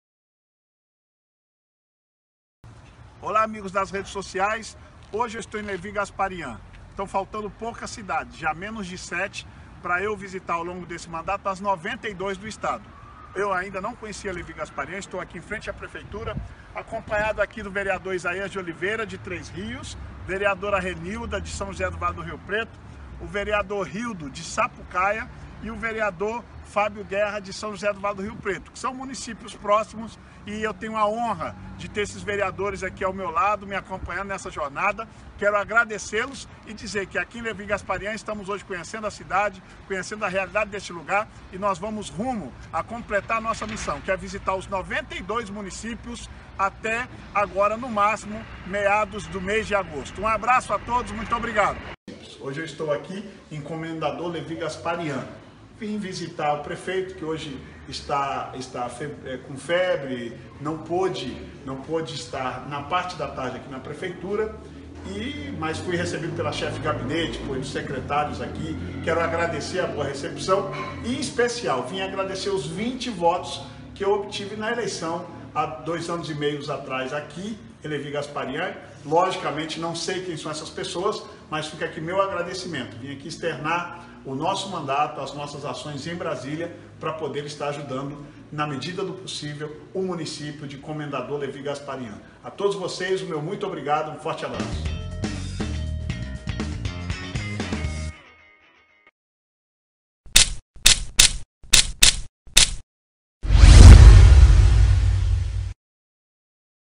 Nos Municípios: Comendador Levy Gasparian RJ
Minha visita a Comendador Levy Gasparian-RJ, para agradecer os 20 votos que tivemos nesse município.